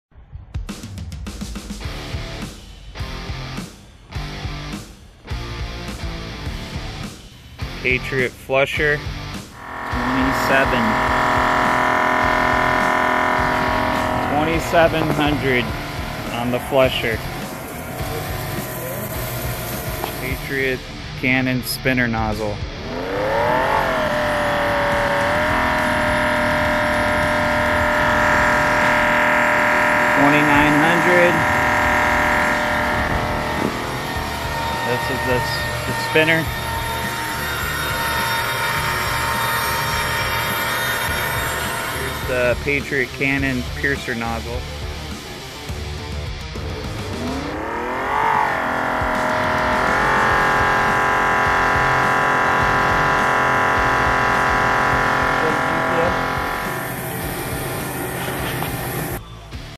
Patriot Cannon Mini Jetter - Pressure & GPM
Running the Patriot Cannon Mini Jetter and testing the PSI / GPM of included nozzles.